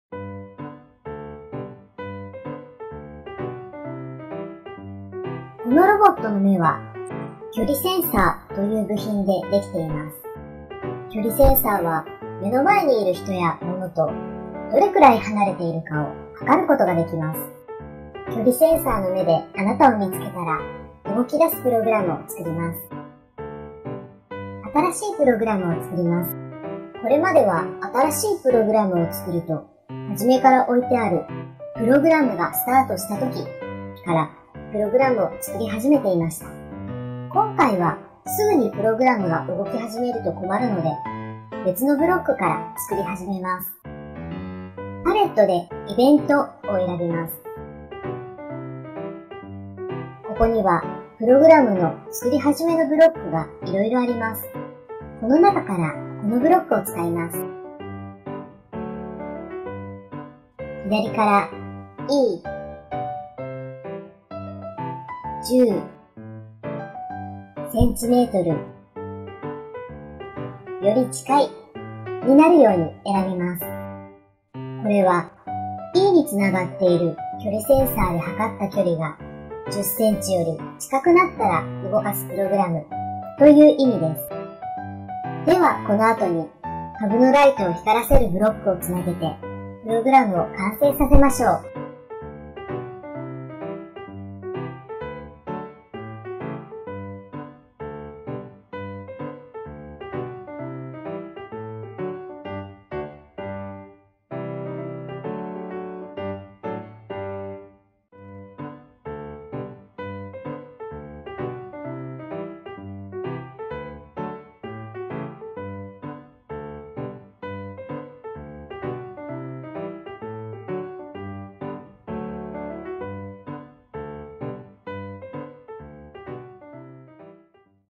【プログラミング学習教材「プレフルラボ with レゴ SPIKE」】「プログラミングのきほん」解説動画：みつけよう
▼家庭向けプログラミング学習学習教材『プレフルラボ！with レゴ SPIKE』とは？
「『Playful（ワクワク夢中になる）な学び』を通して、子どもたちの自由な創造力を引き出し、無限のアイデアを拡げたい」という想いでレゴ®エデュケーションのロボットプログラミング教材「SPIKEプライム」を使った家庭向けプログラミング学習教材をご用意しました！

▼解説動画について
ロボットが動いている様子やプログラミング方法のアドバイスの動画です。
課題が終わったあとの答え合わせてとして活用できますし、課題が難しかった場合にヒントとして活用することもできます。